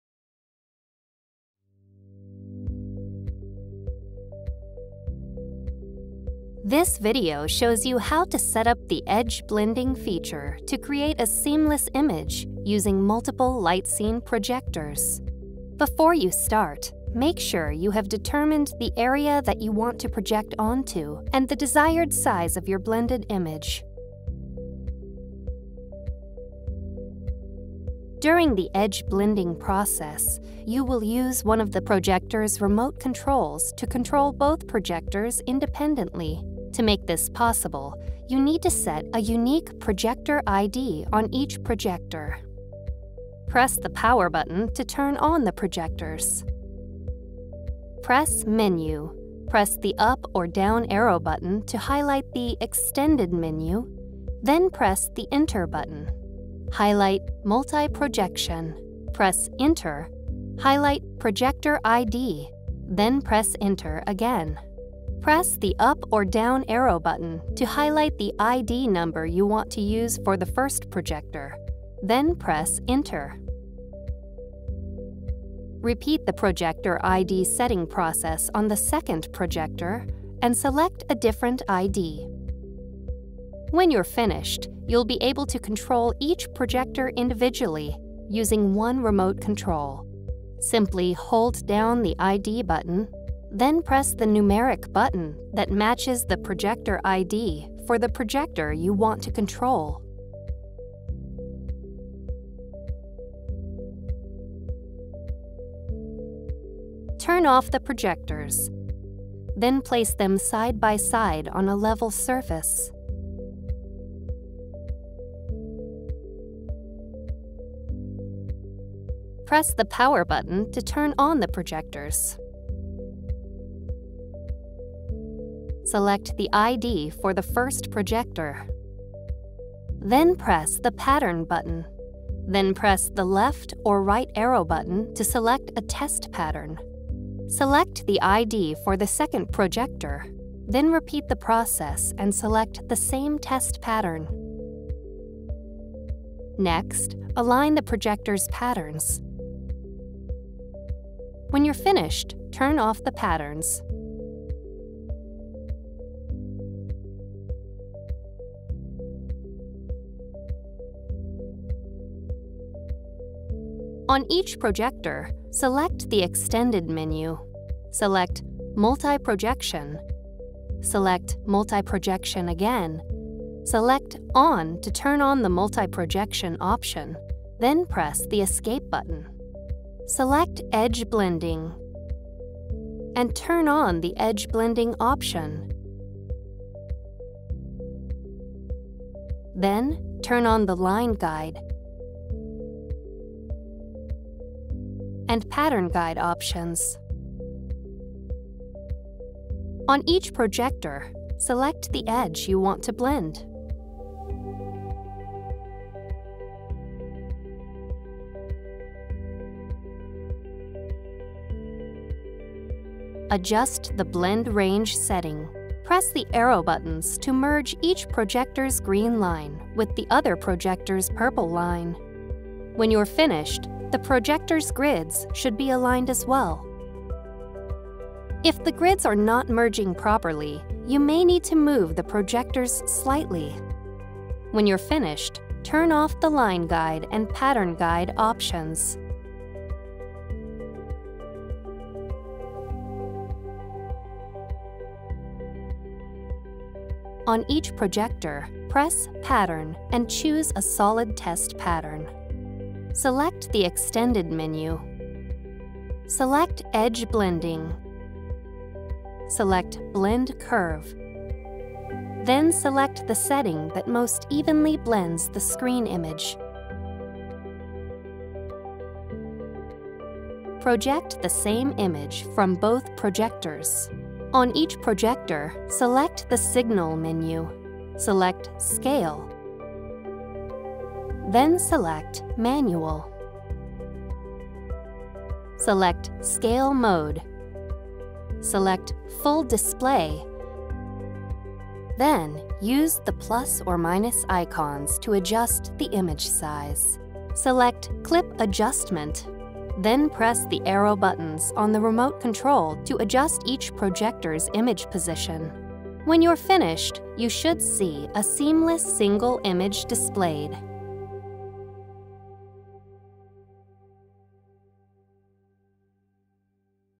Epson LightScene EV-100/EV-105 | Edge Blending Setup
Learn how to use Edge Blending to combine images from multiple Epson LightScene EV-100/EV-105 3LCD laser projectors.

Questions? (Canada) and search for your product.